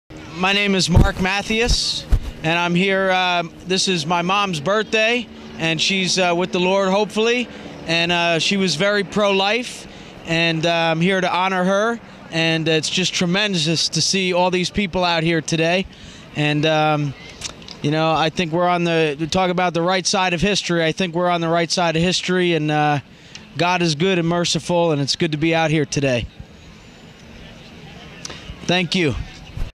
Mark Sacrifices for his Mother at the March For Life
Mark Mathies of Spirit and Truth in Lyndon , New Jersey  dedicated his sacrifice of attending the March For Life to the memory of his deceased mother on her birthday.